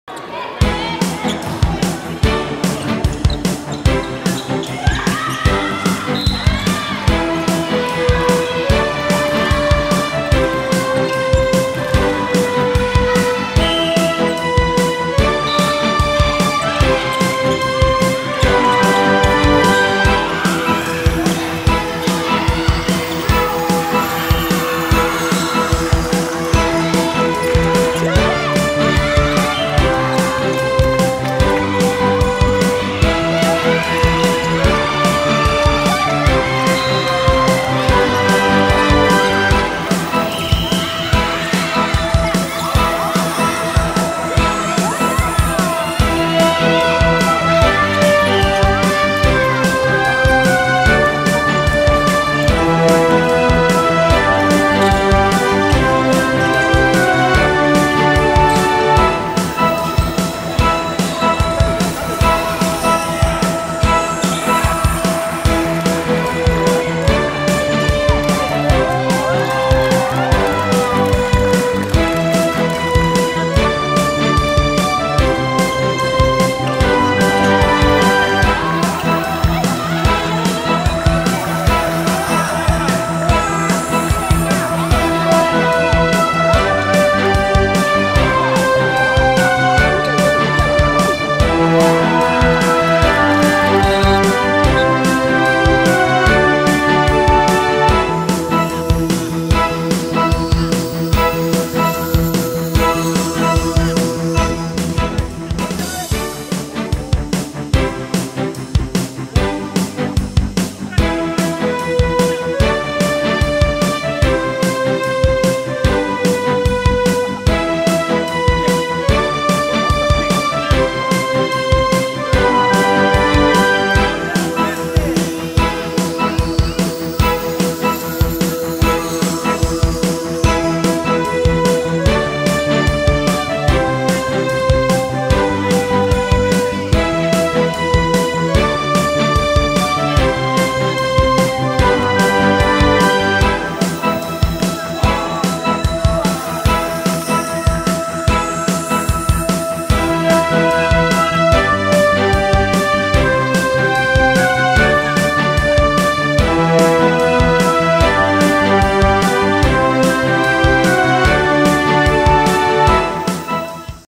第６４回石川県民体育大会夏季大会（羽咋郡市）
平成２４年に羽咋市で行われた種目のうち、８月１１日（土）に開催したバレーボールとバスケットボール、サッカーの一部映像です。